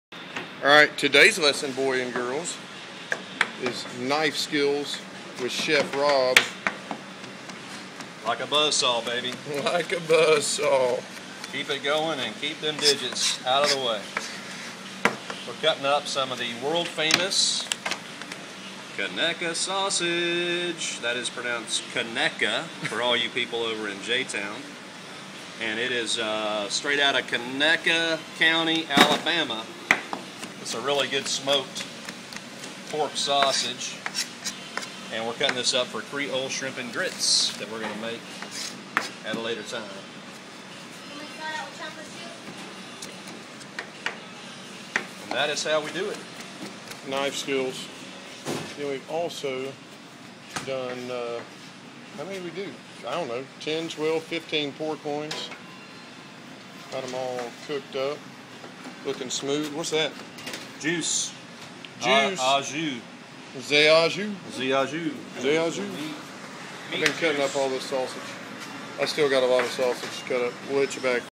Knife skills with chef rob... That boy is a ninja!!!
Mad ninja skills being displayed on some unsuspecting sausages...